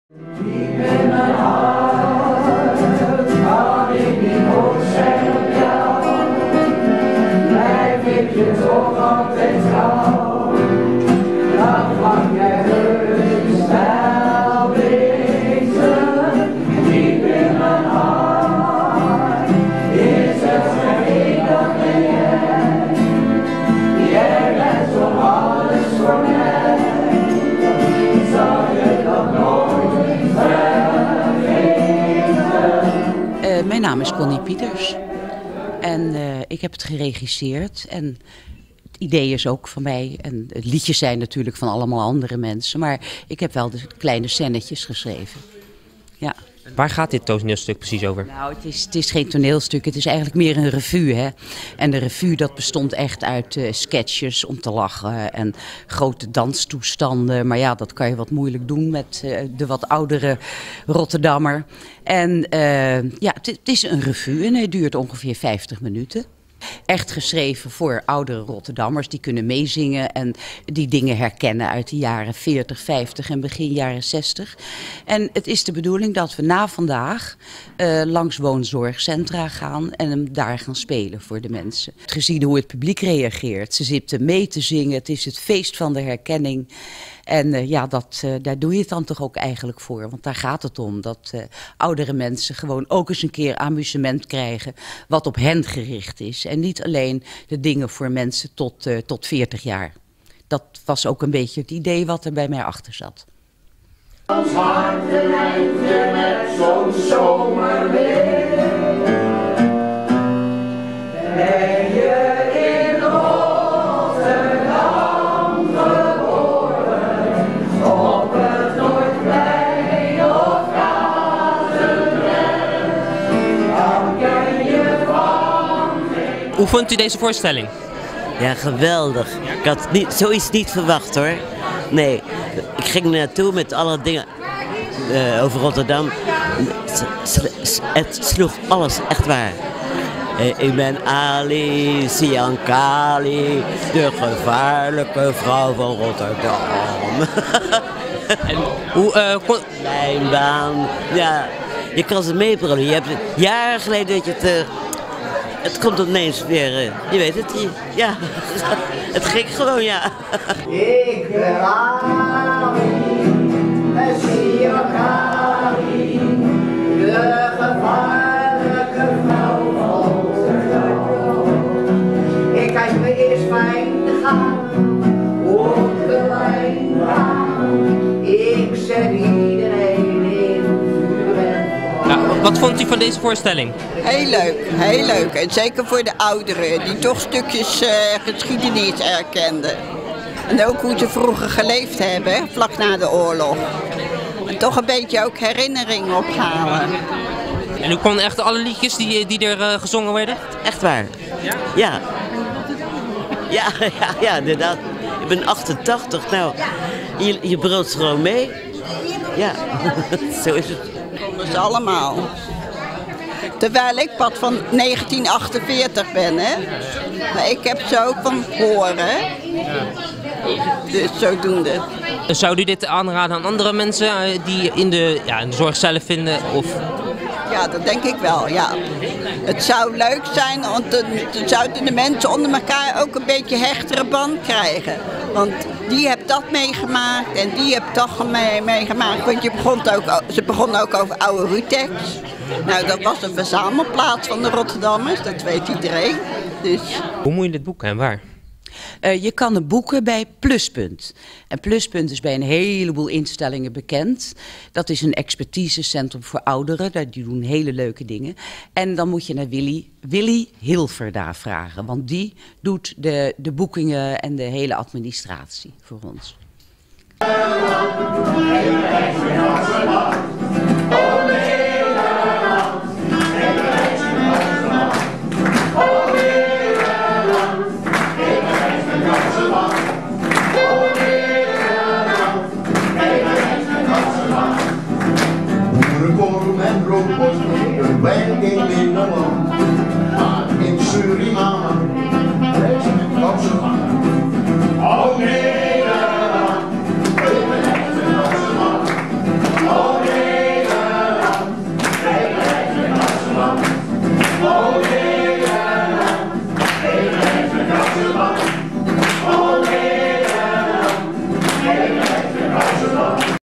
Cineac Noord - Ouderenrevue de Wederopbouw
Ouderenrevue de Wederopbouw is goed voor het ophalen van het geheugen en te boeken via Pluspunt Rotterdam.